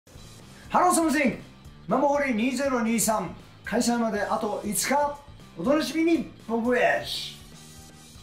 開催まであと5日！『ママホリ2023 ～Genking Live～』
今回のキーワードは“ファミリーの休日”です。
日頃、ホッと息をつく暇もないママとパパたちに、自分自身もママを経験してきた、今も現役バリバリの人気女性アーティストにしか出来ないパフォーマンスを通して”心の休日”を楽しんでもらいたい！
『ママホリ2023』は、ママとファミリーの輝きをフルチャージする、素敵な時を提供します！

【公演名】
ママホリ2023 ～Genking Live～

【日時】
2023年10月9日(月・祝)
14:00開場/15:00開演

【会場】
TACHIKAWA STAGE GARDEN（立川ステージガーデン）

【出演者】
ナビゲーター：ダイアモンドユカイ

【チケット】
一般(26歳以上)：8,800円（税込）
高校生以上～25歳以下：4,400円（税込）
小中学生：2,200円（税込）
※未就学児童は、保護者1名につき、膝上観覧1名まで無料
※25歳以下の方は、公演当日にお名前が確認できる公的な身分証明書を必ずご持参ください。

【主催】
WOWOWプラス

【お問合せ先】
キョードー横浜
(土日・祝を除く、平日11:00〜15:00)